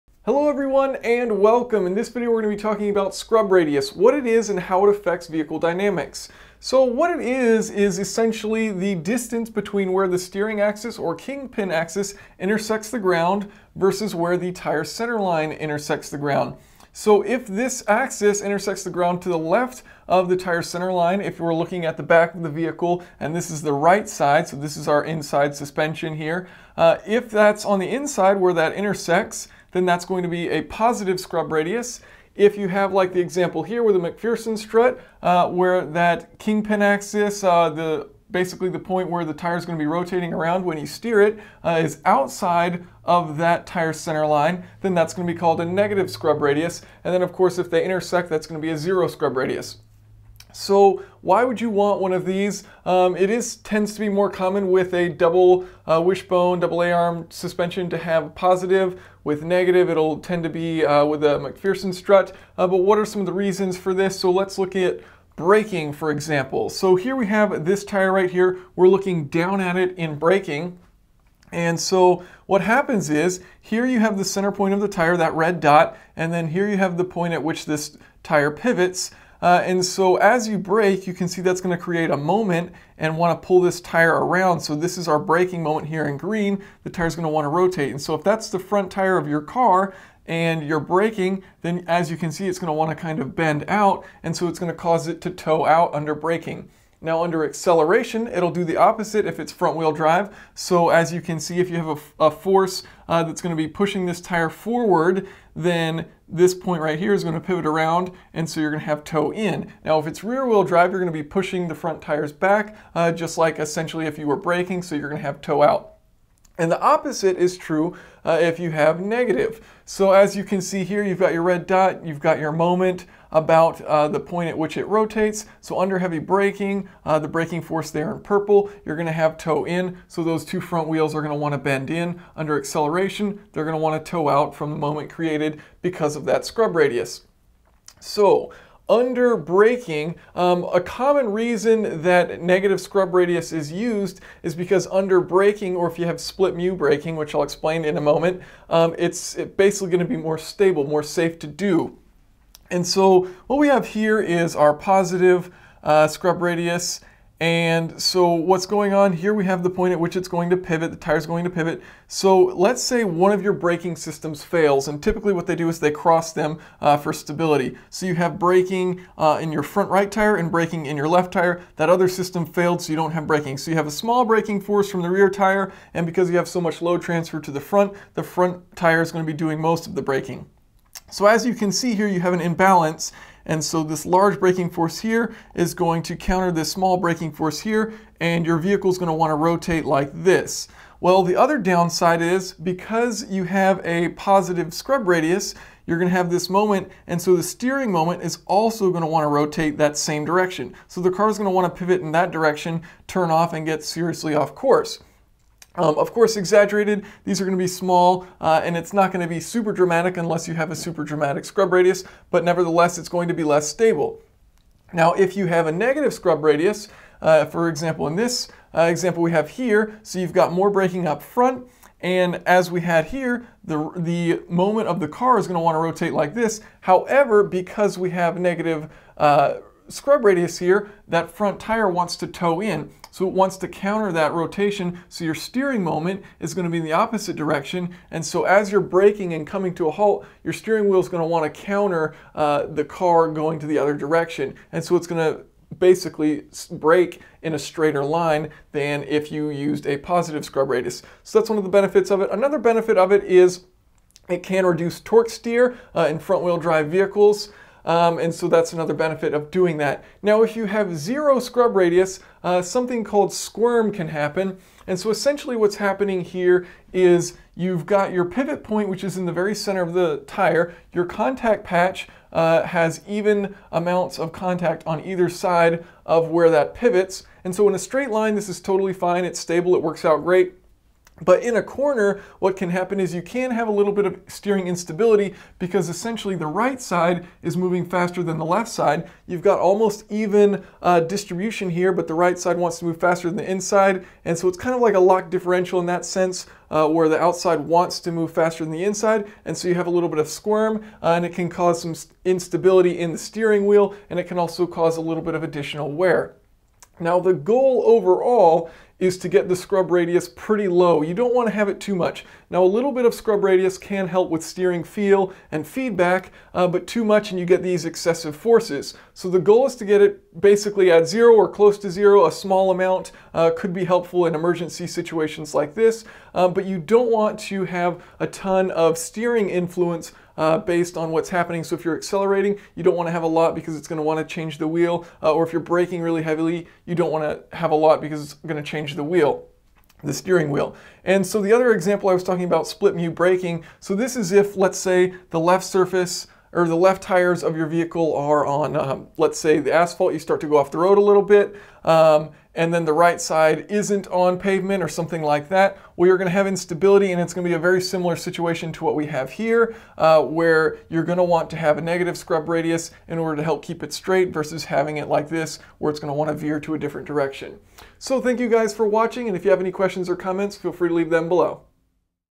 Scrub Radius - Suspension Design - Explained
The scrub radius of your car can be altered with different wheel offset and suspension design. This will affect how the tire contact patch interacts with the road during cornering, braking, and acceleration, and can cause unique toe characteristics depending on the design. A positive scrub radius tends to be beneficial while braking into a turn. A negative scrub radius tends to better for braking stability in the event of brake failure or split-mu braking. Typically the scrub radius is configured to be as small as possible to minimize the effects under braking, but squirm (from zero scrub radius) is also undesirable.